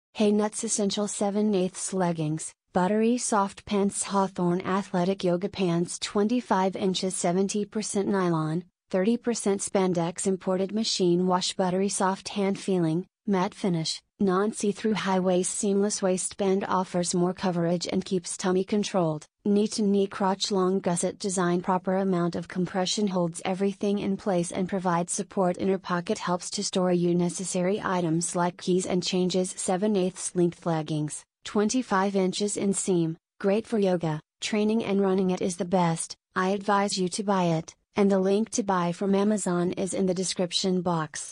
HeyNuts Essential Leggings
70% Nylon, 30% Spandex
Imported
Machine Wash
Buttery soft hand feeling, matte finish, non see-through
High waist seamless waistband offers more coverage and keeps tummy controlled, knee to knee crotch long gusset design
Proper amount of compression holds everything in place and provide support
Inner pocket helps to store you necessary items like keys and changes
7/8 length leggings, 25'' inseam, great for yoga, training and running